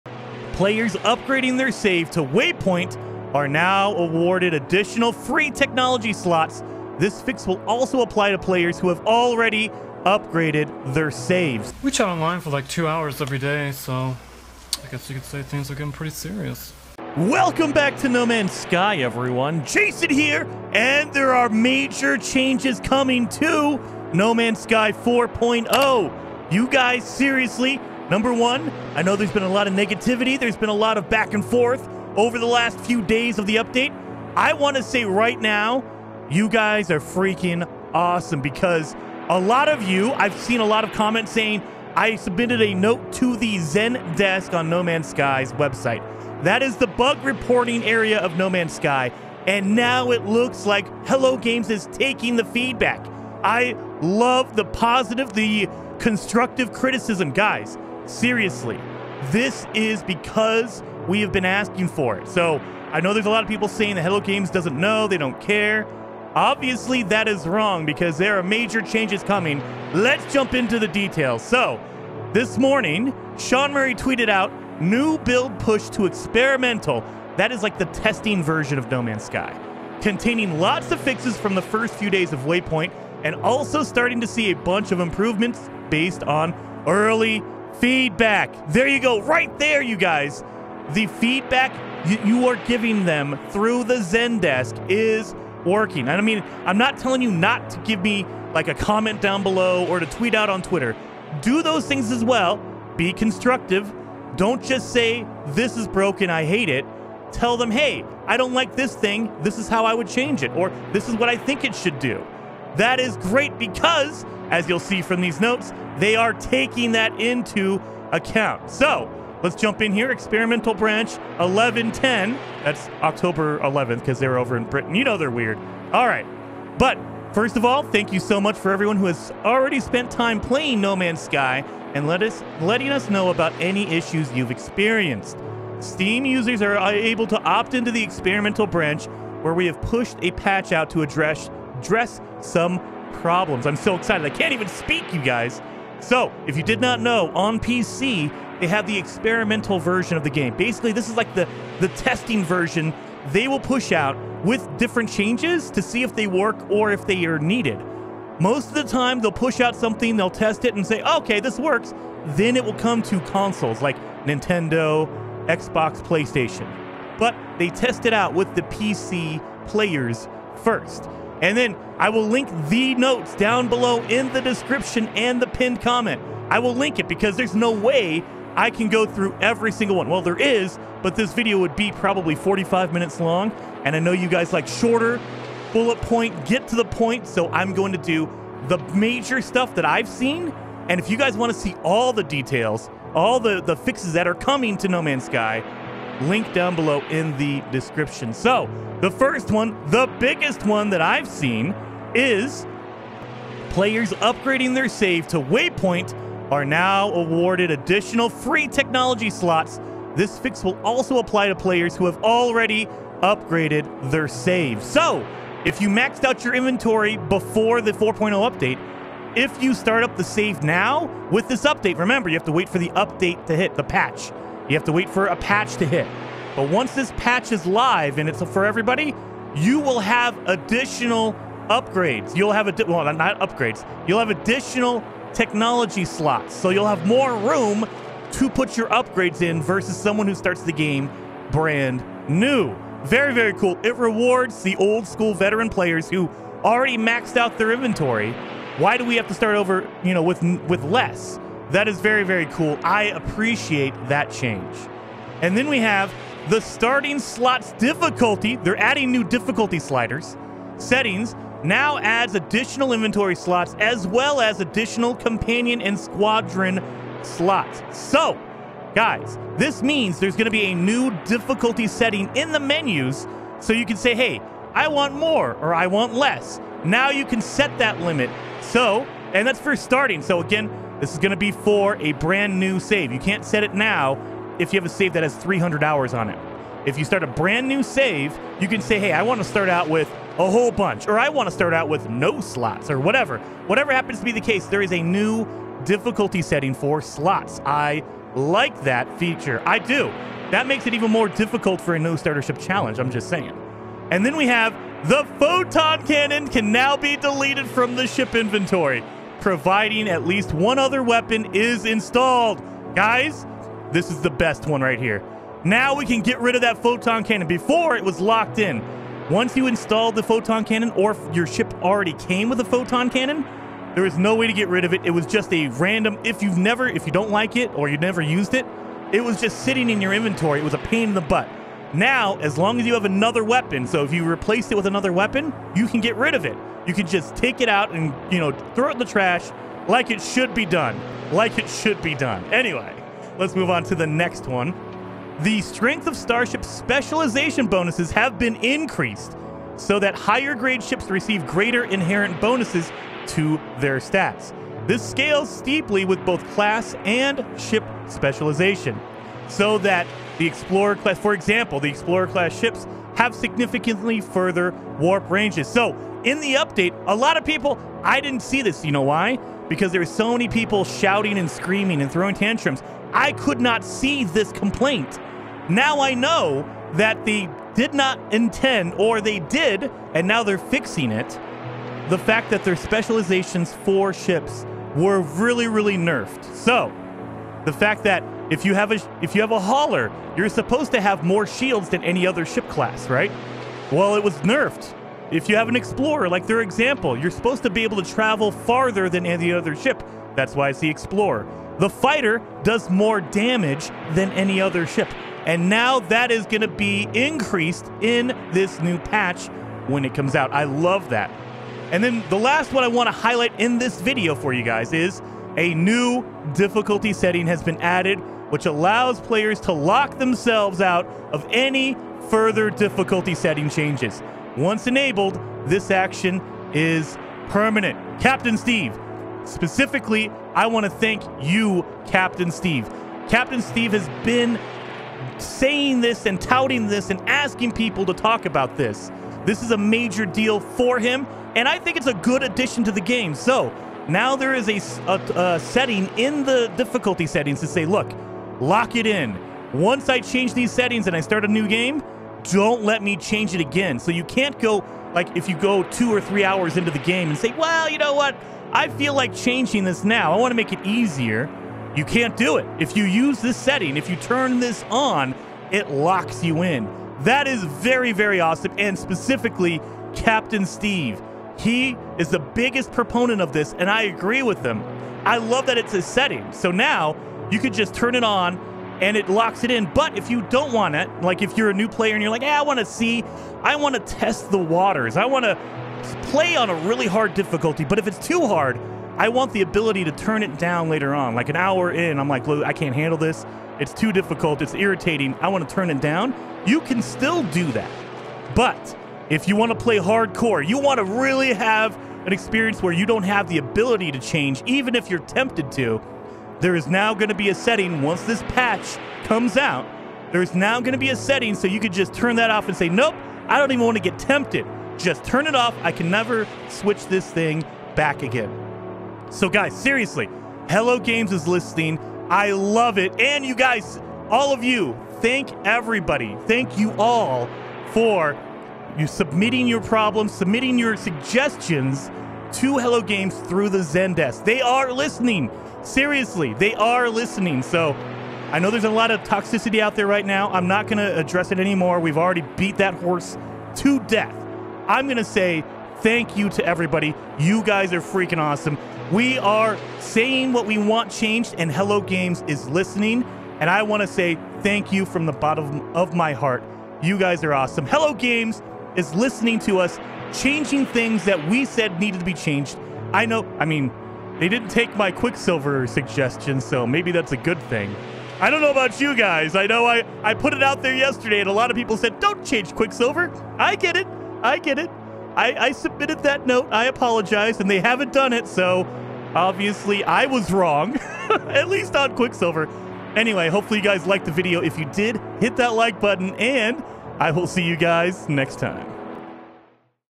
Major Changes Coming! No Man's Sky 4.0 Waypoint Update
Major changes! There are major changes coming to No Man's Sky in the new patch! We have the new patch notes for No Man's Sky 4.0 and they are super awesome! New game difficulty settings, inventory changes, buffs to ships, a ton of changes based on our feedback! Thank you so much to everyone submitting bug reports and constructive feedback to Hello Games. They hear us and they are making changes to the game!

My PC Specs:
64GB G.Skill Ram
Samsung 970 EVO Plus 2TB